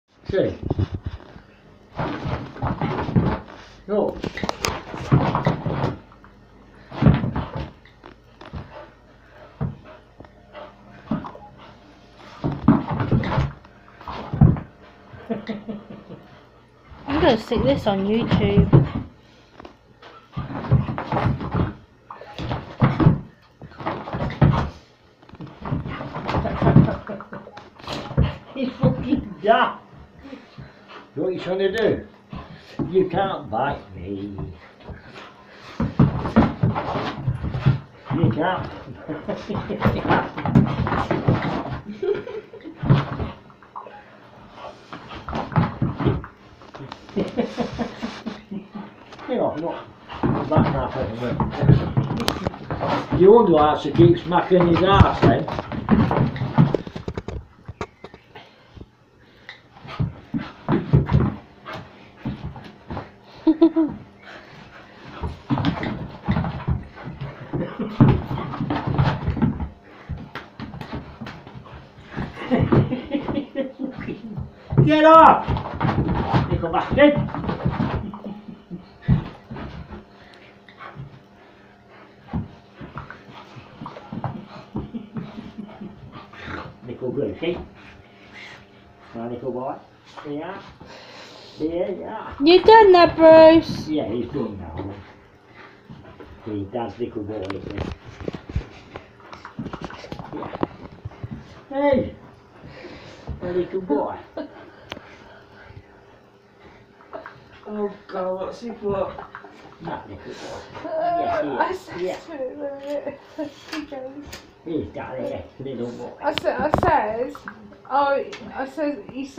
Dogs Brucie and Grandpa
My grandpa playing with Brucie on my birthday 17/4/16... Brucie was on a mad one.